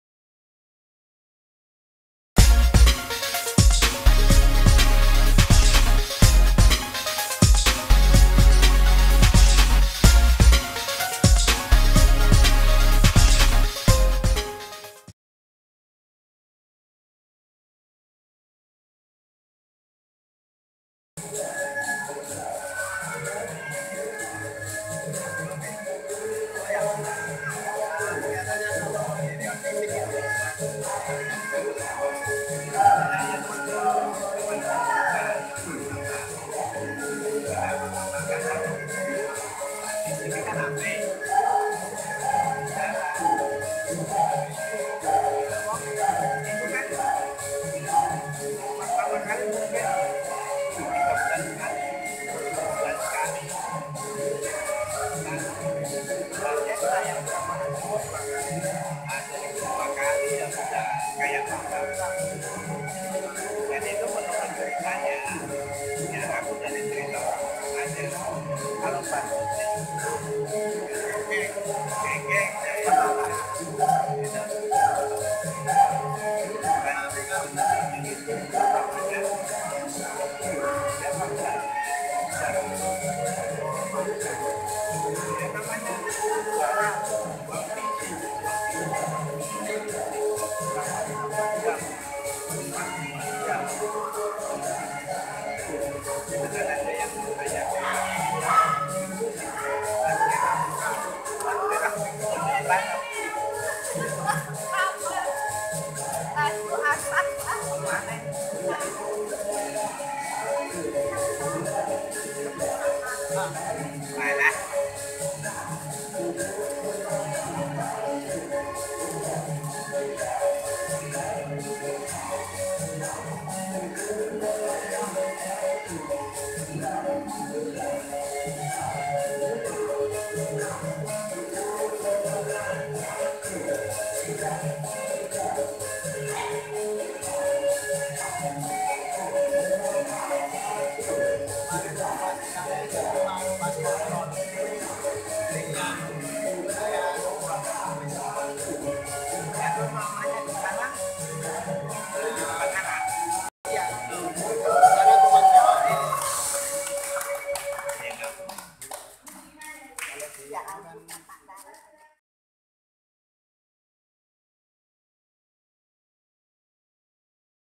SENAM LANSIA GKP SEDAYU
"NIKMATI HIDUPMU HARI INI, SELAGI SEMPAT... SEBAB ESOK HARI, BELUM TENTU SEGALANYA MASIH AKAN SAMA"

Kelompok senam para bapak-ibu usia lanjut (lansia) ini dirintis oleh seorang pensiunan tentara, namanya Pak Koencoro. Pernah 5 tahun menjadi anggota Paspampres di era kepemimpinan Soeharto.